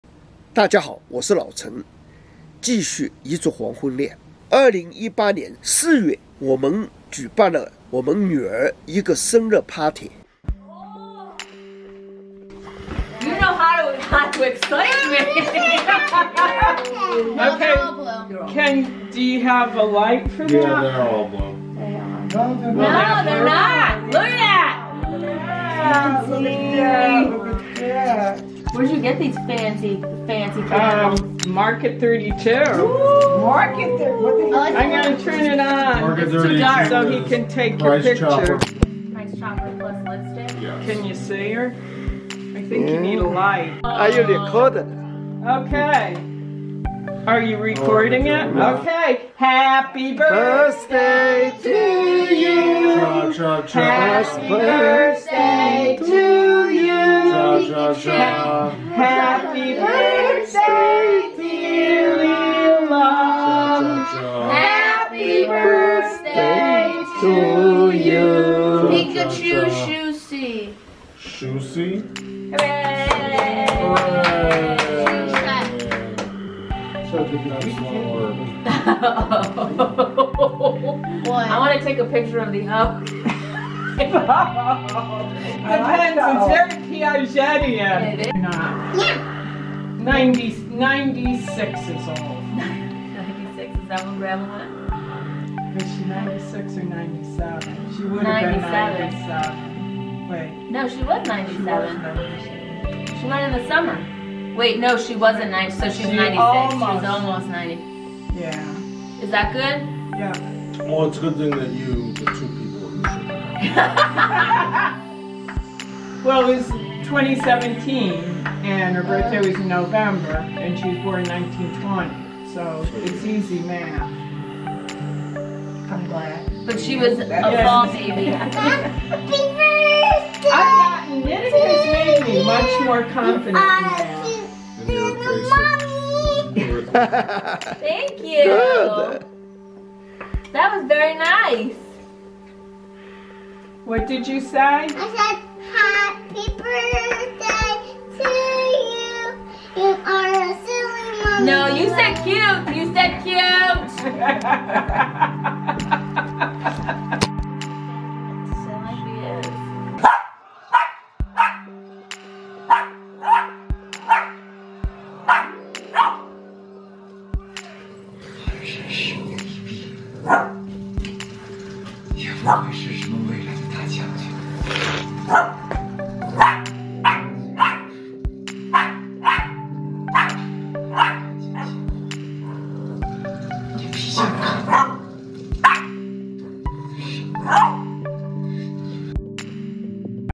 Our Daughter's Birthday Party(Interracial Sunset Love Story #35)
在4月我們家庭聚會慶祝我們的女兒生日快樂！

Please also watch:
Why did I leave from China？Playlists【My Working Life During 2 Dacades】#1
Meeting in Cafe。Playlists【 (Interracious Sunset Love Story 】#1